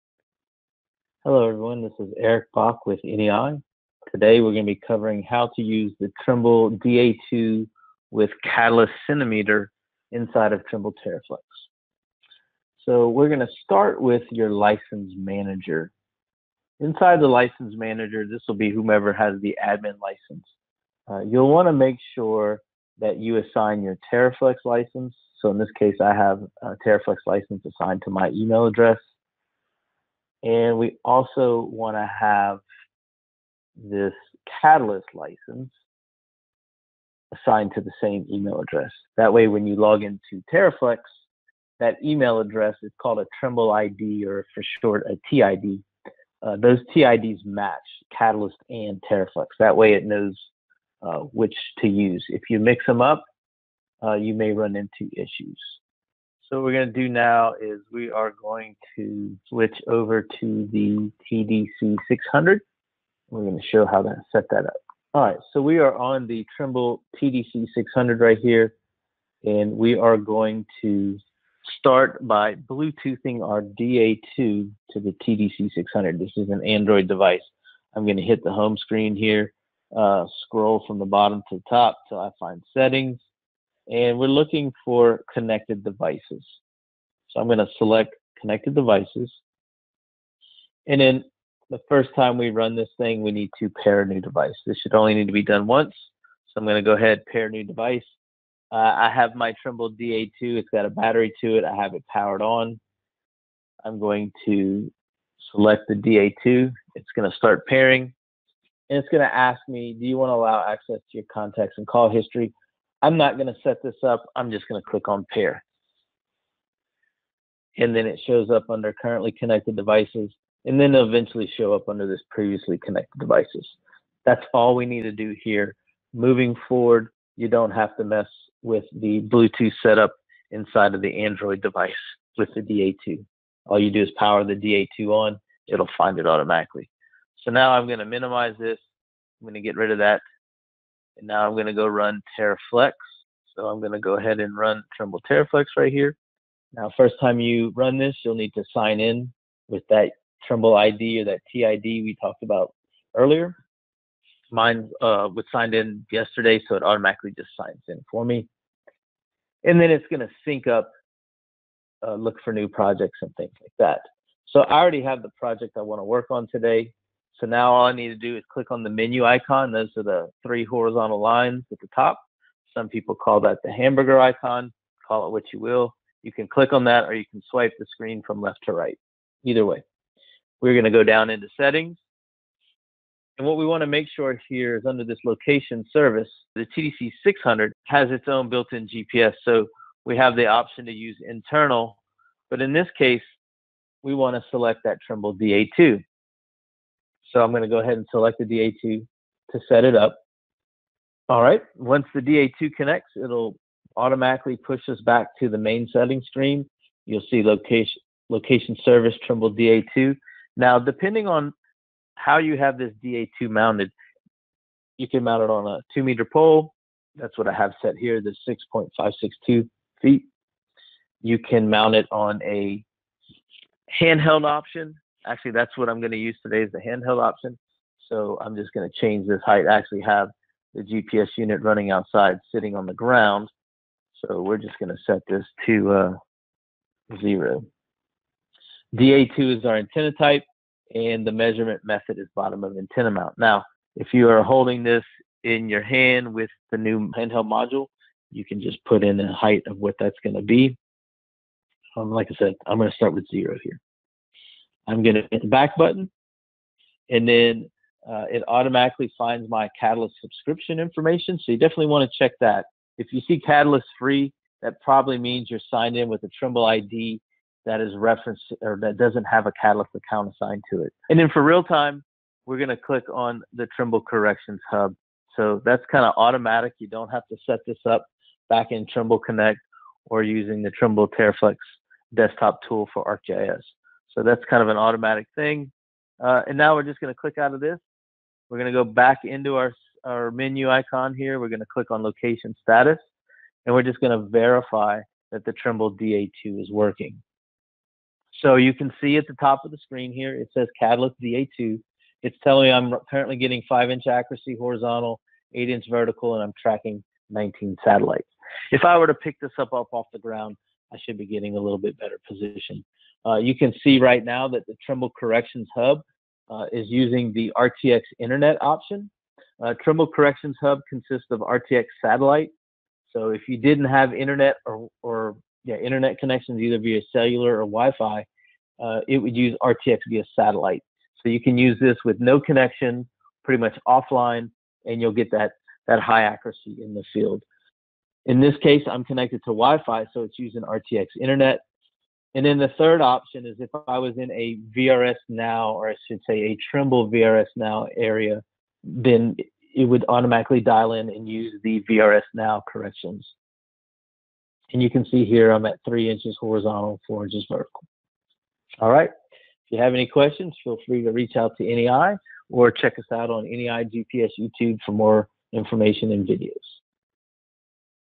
Using Trimble DA2 GNSS Receiver in TerraFlex
In this video Eric Bock from NEI will discuss how to configure the Trimble DA2 GNSS receiver with Catalyst 1 real-time corrections inside of Trimble's Mobile Field data collection application.